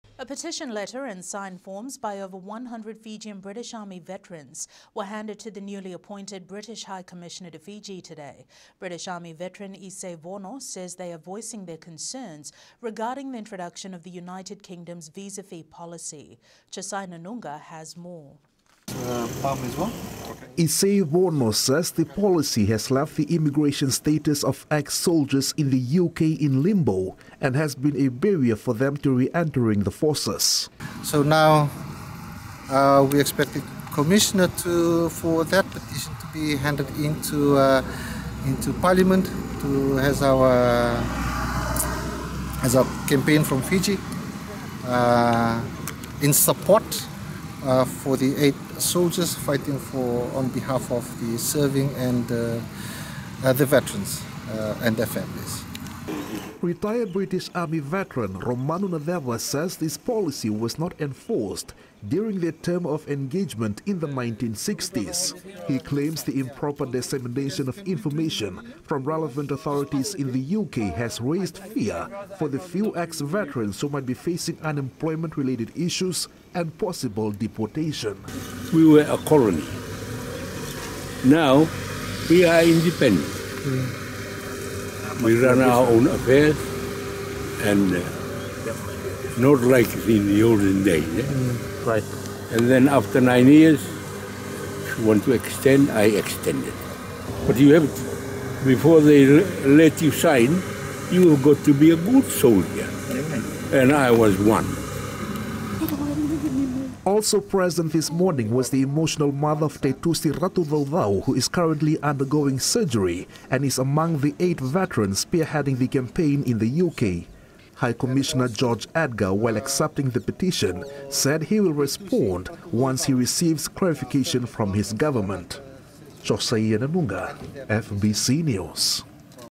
Fijian British Army Veterans Deliver Petition to British High Commissioner, Suva, Fiji.
Despite attempts made by the Fiji Police to stop our campaign for Justice march to "Abolish the UK Visa Fee's Policy" at the very last minute, we still managed to deliver our 115 signed petition letter to the British High Commissioner, Mr Christopher George Edger.

Team CNV did all we could to make sure we followed what was asked of us from all government departments in Fiji we were advised to approach to get approval prior to the protest march.

 Public announcements were paid and advertised in the local newspapers, Road Authority gave their permission for the demonstration to take place and other local authorities informed.

However, Police Headquarters handed us our decline notice to march, at the 11th hour of the day before our march.

 They even turned up at the assembling point in Sukuna Park, to stop our veterans from gathering in large numbers which was very sad to see baring in mind that the Fiji sevens coach, Gareth Baber,  was live on Facebook yesterday afternoon filming children playing seven's.

The police officers informed us that we were not allowed more than 3 people to gather in public, yet the public are allowed to watch rugby tournaments almost every weekend!!!

 We were not disheartened and we continued  true and dedicated to our commitments we were on the day and as a result we achieved our end goal.

Big thank you to all those veterans, families and supporters who turned up at Sukuna park, Suva last night, Fiji time (Wed 30th September) and was told to return home.

Your support and belief in us meant a great deal. You put yourselves at risk regardless and we commend you all for your bravery.

Thank you Isei Vono for leading our team and for not giving up despite the numerous hurdles you've had to overcome.  Plan A-C well executed.

Thank you Mrs Ratucaucau for being there and for speaking up for your son, Taitusi Ratucaucau.

 Last but not least, vinaka vakalevu Mr. Romanu Naceva MBE for turning up like a true soldier that you are. Heart wrenching moment when you insisted on joining us at our campaign even though you weren't feeling too well. Spoken like a true patriot, "Once a gunner always a gunner"

Eighty year old Mr Romanu Naceva MBE,  26 Field Regiment Royal Artillery was recruited in Fiji in Nov 1961. One of the first 212 Fijian men to be recruited in the British army in the 1960's.

Mr Naceva confirmed that he along with his fellow colleagues didn't pay a single penny towards getting their  citizenship after coming out of the army. He served 22 years in the British army.

 We at team CNV are the leading advocates for justice and fair treatment of our armed forces community. We will not give up that easily and we will continue to be on the frontline of this whole campaign.